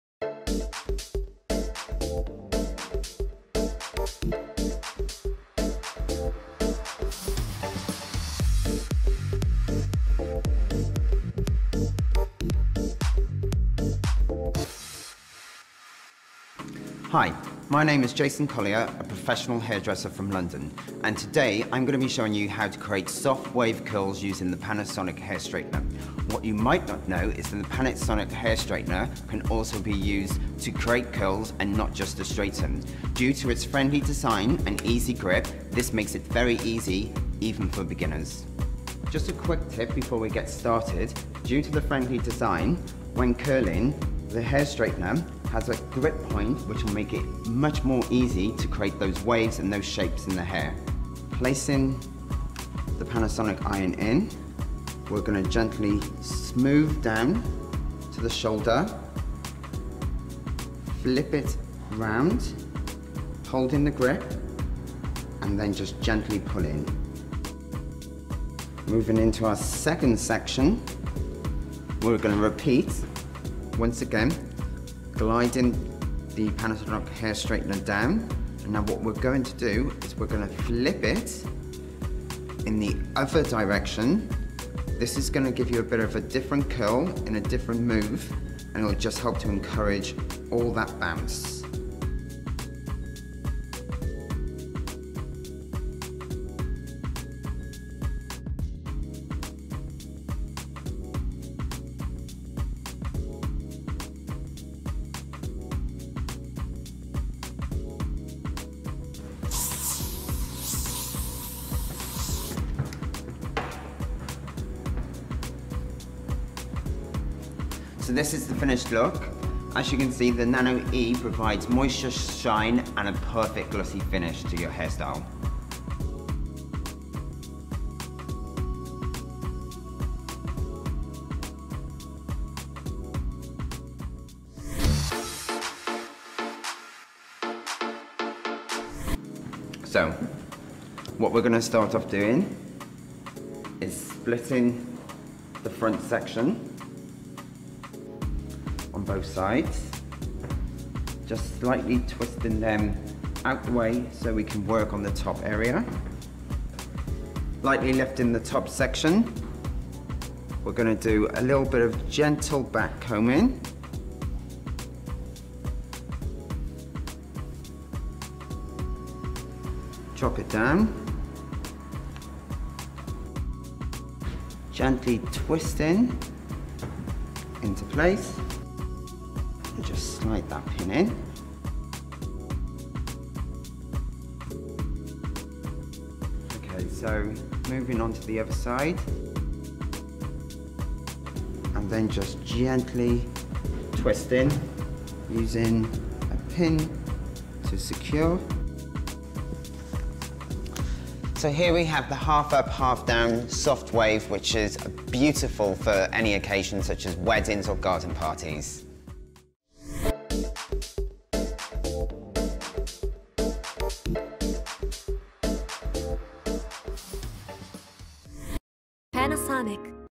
Hair tutorials oleh Jason Collier dengan EH HS99 | SOFT CURLS TWISTED HALF UP
Rambut ikal dan bouncy curls cocok untuk gaya feminin dan romantis. Penampilan yang anggun sekaligus kasual ini sempurna untuk aktivitas sehari-hari dan juga acara istimewa.
Coba gaya rambut ikal cantik ini menggunakan Panasonic Hair Straightener EH-HS99 dengan tutorial dari hair stylist profesional, Jason Collier.